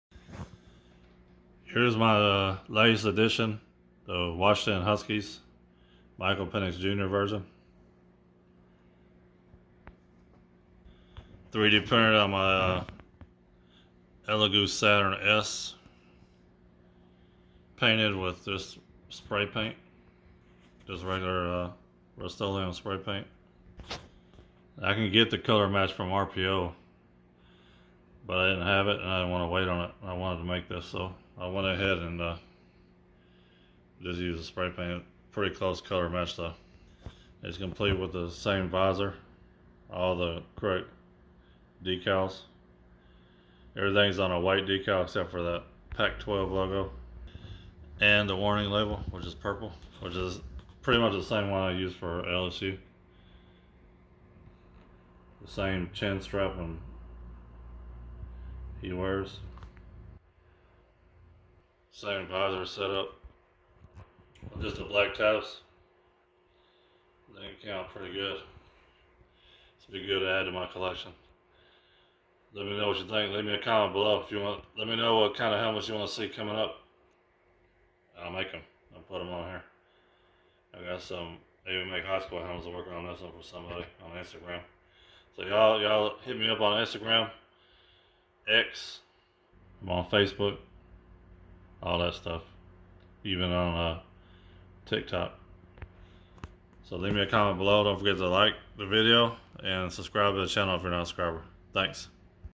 Washington Huskies 3d printed speedflex pocket pro
I made this Washington Huskies Speedflex on my Elegoo Saturn S. Hand painted and custom made water slide decals. Added a Oakley visor as well.